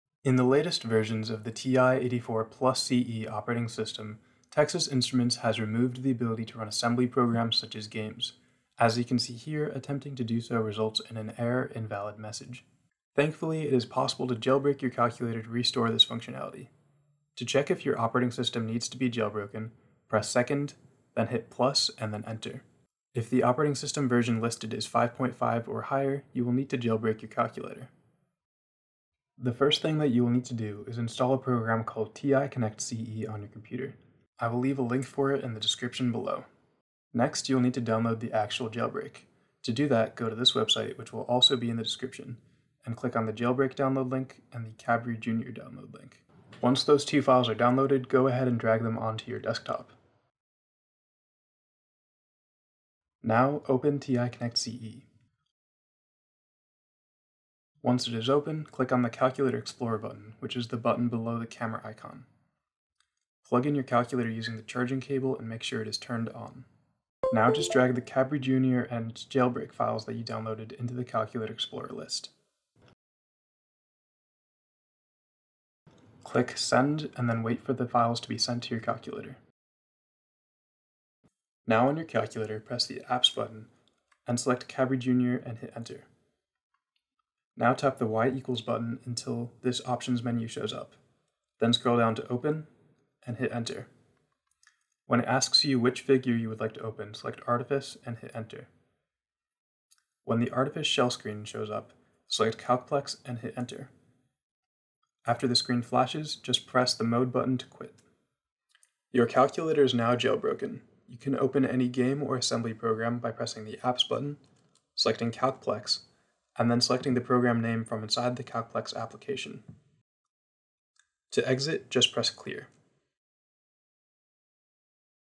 How to Jailbreak the TI-84 Plus CE
Version 5.5 of the TI-84 Plus CE operating system removes the ability to run assembly programs/games - causing issues such as "Asm(" being hidden and Error: Invalid when trying to run programs. Fortunately, it is possible to restore this functionality by jailbreaking your calculator using an exploit called arTIfiCE.